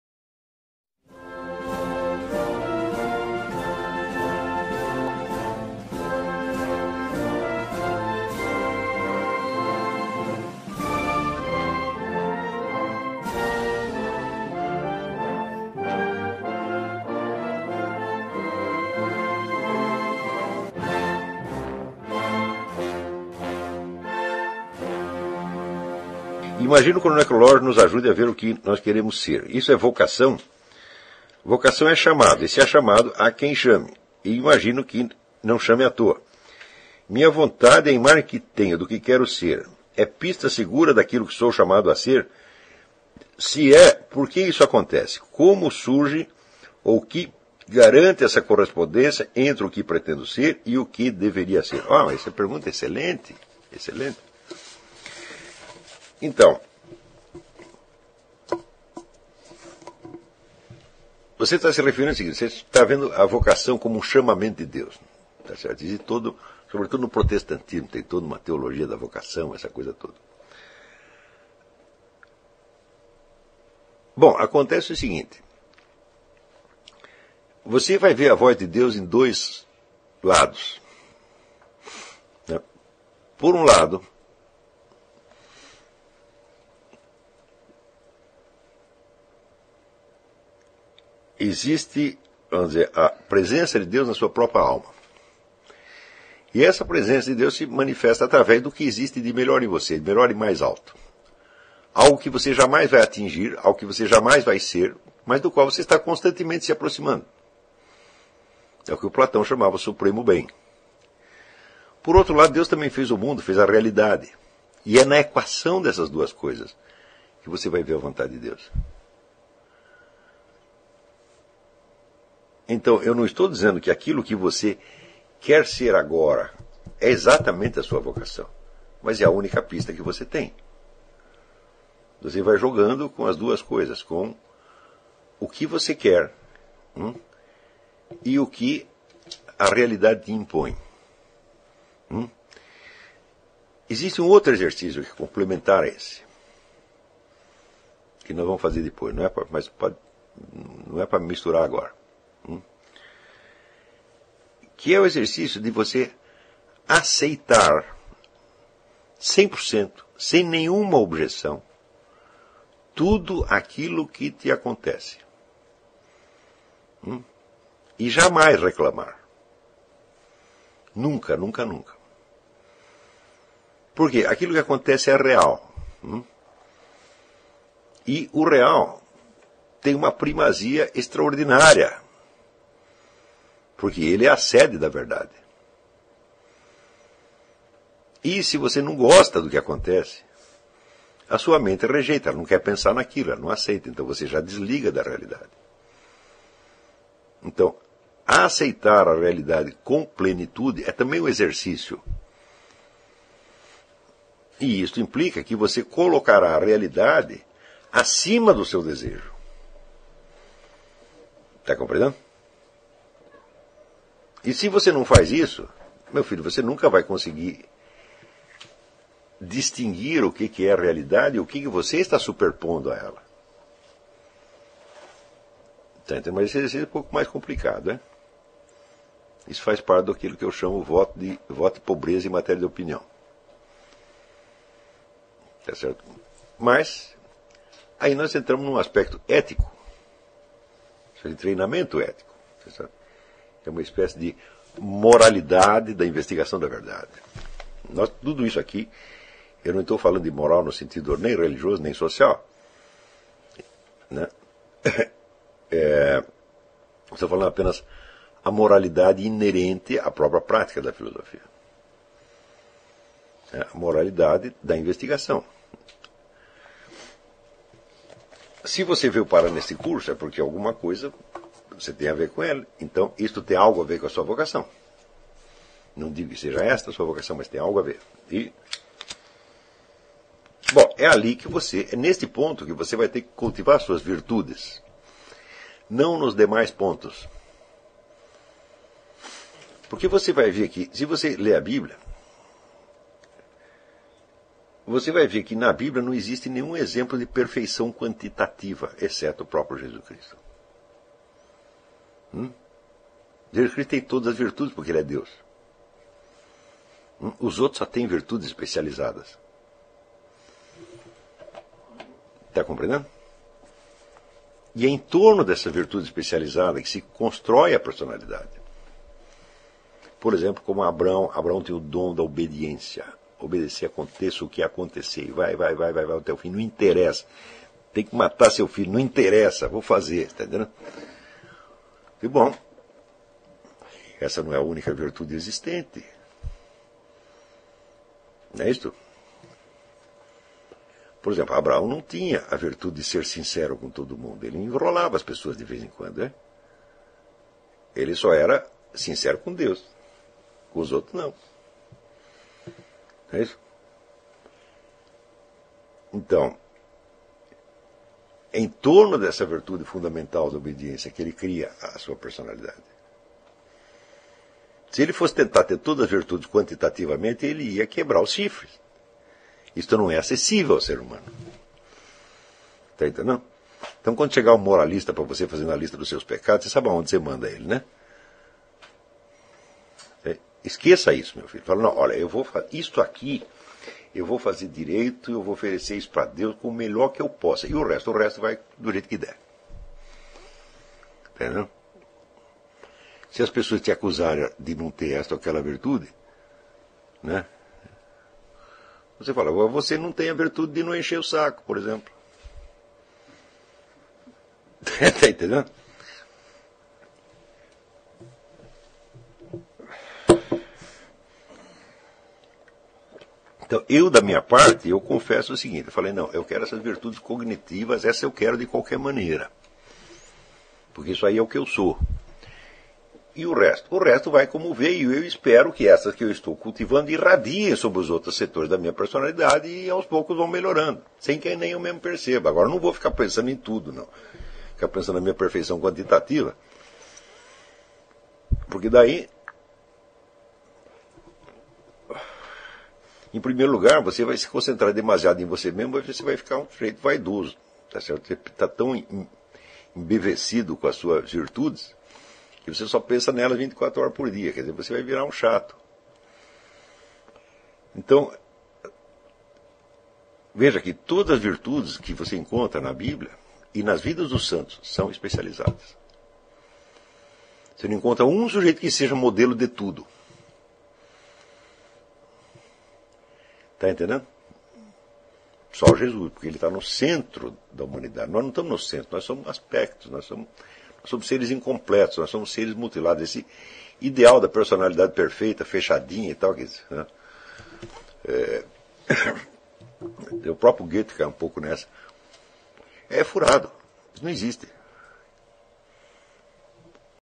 Olavo de Carvalho - O que são Vocação e Virtude?
Professor Olavo de Carvalho discorrendo e explicando sobre Vocação e Virtude.